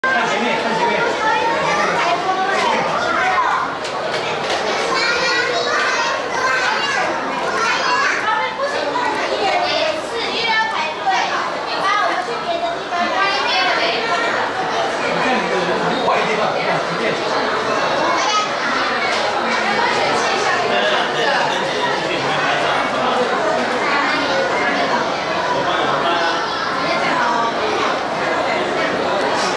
2015.7.1 中央氣象局74週年局慶開放參觀活動 生活有氣象之氣象與交通特展
2015.7.1 中央氣象局74週年局慶開放參觀活動 生活有氣象之氣象與交通特展